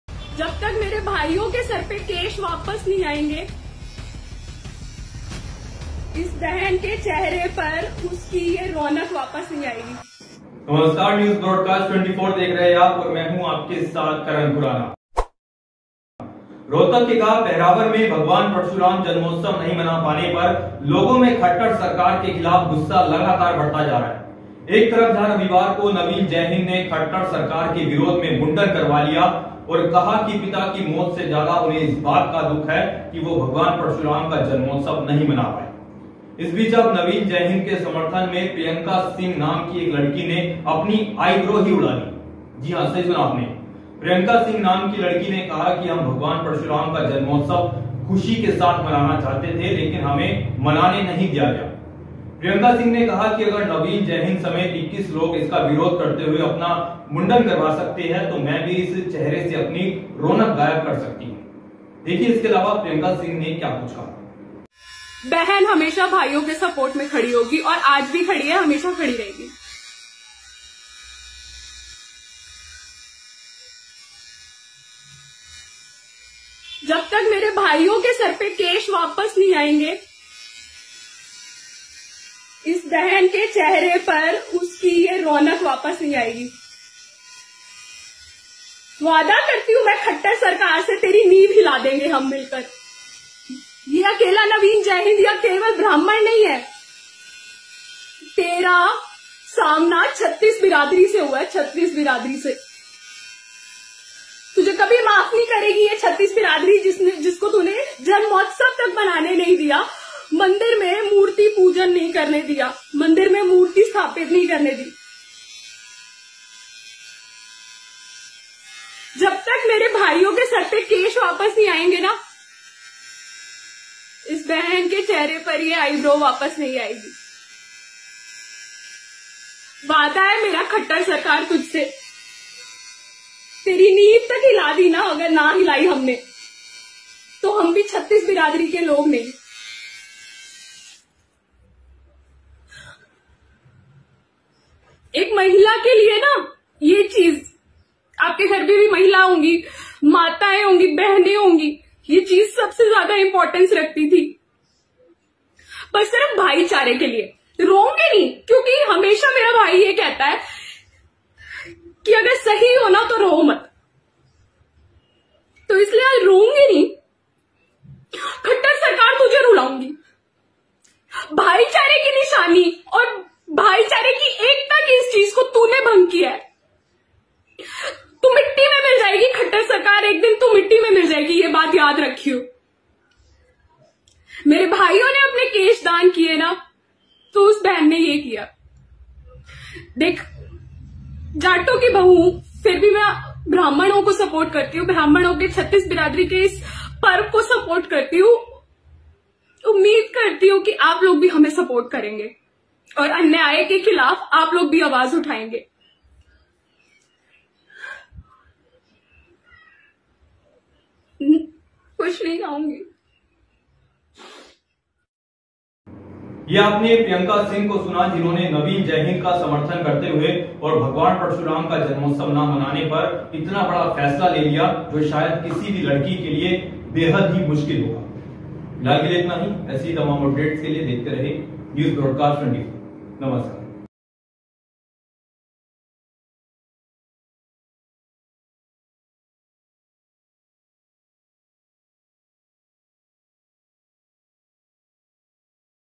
भगवान परशुराम जन्मोत्सव को लेकर बढ़ा बवाल, अब इस लड़की ने उड़ाई अपनी आइब्रो
रोहतक के गांव पहरावर में भगवान परशुराम का जन्मोत्सव नहीं मना पाने पर लोगों में खट्टर सरकार के खिलाफ गुस्सा लगातार बढ़ता जा रहा है| एक तरफ जहां रविवार को नवीन जयहिंद ने खट्टर सरकार के विरोध में मुंडन करवा लिया और कहा कि पिता की मौत से ज्यादा उन्हें इस बात का दुख है कि वो भगवान परशुराम जन्मोत्सव नहीं मना पाए। इस बीच अब नवीन जयहिंद के समर्थन में प्रियंका सिंह नाम की लड़की ने अपनी आइब्रो उड़ा दी. जी हां, दरअसल, प्रियंका सिंह ने कहा कि अगर नवीन जयहिंद समेत 21 लोग इसका विरोध करते हुए अपना मुंडन करवा सकते है| तो, मैं भी इस चेहरे से अपनी रोनक गायब कर सकती हूं|

Anger against the Khattar government is increasing among the people for not being able to celebrate the birth anniversary of Lord Parshuram in Pahrawar village of Rohtak. On the one hand, on Sunday, Naveen Jaihind got himself shaved in protest against the Khattar government and said that he was more sad that he could not celebrate the birth anniversary of Lord Parshuram than his father's death. Meanwhile, in support of Naveen Jaihind, a girl named Priyanka Singh plucked her eyebrows. Yes, in fact, Priyanka Singh said that if 21 people, including Naveen Jaihind, protesting against it, they can get themselves shaved. So, I too can make my glow disappear from this face|